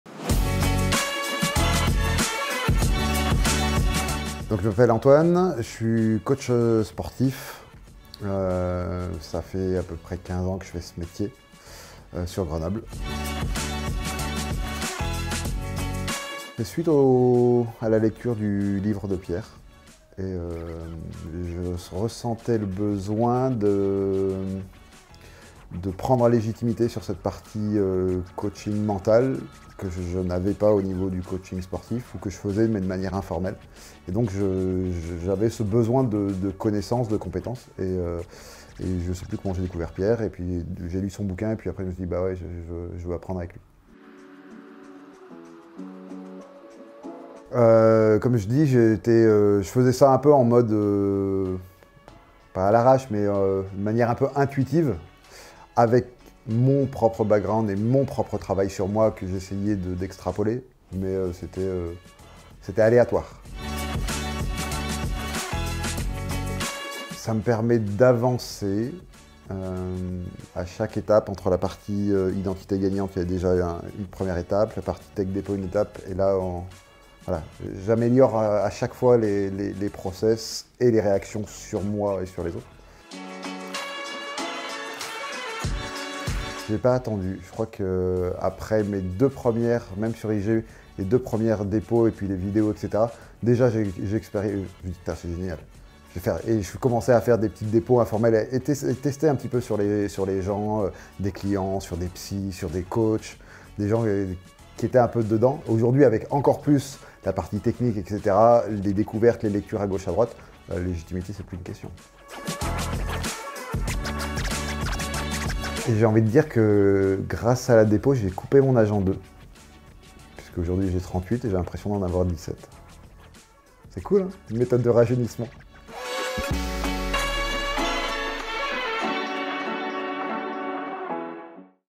Comment devenir un coach mental certifié
Découvrez le témoignage d'Antoine après notre formation Technicien Dépolarisation® :

"C'est génial. La légitimité ce n'est plus une question."